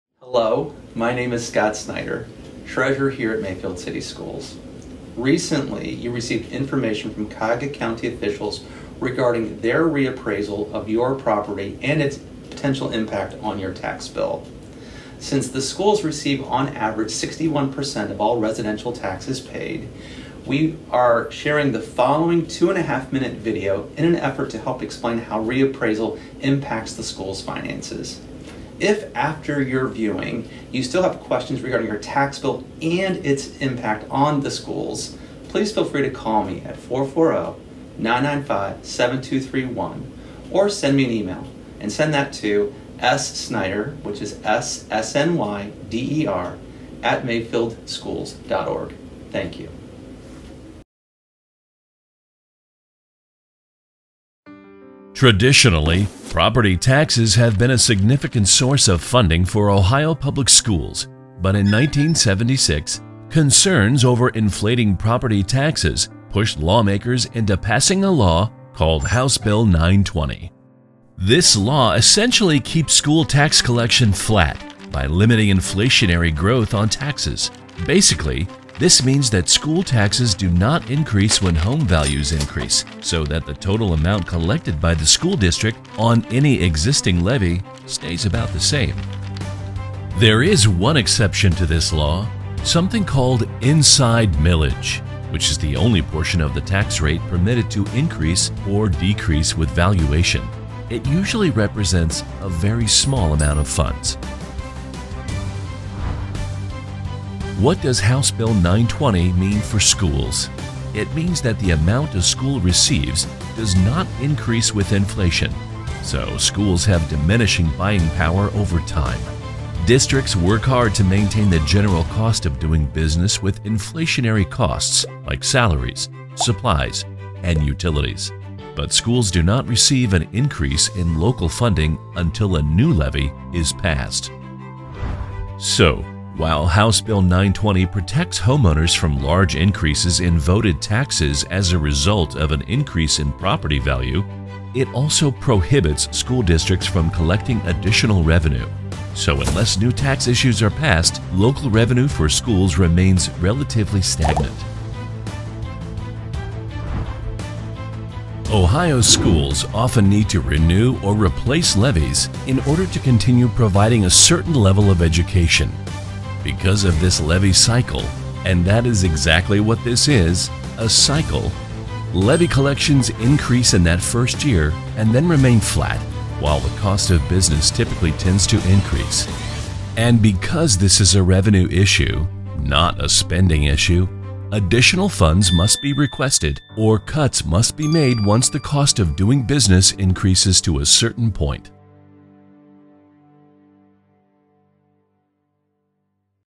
THE FACTS: Mayfield Schools Levy - Treasurer Scott Snyder explains HB 920, reappraisals
Mayfield Schools Treasurer Scott Snyder explains House Bill920, reappraisals and the impact on our schools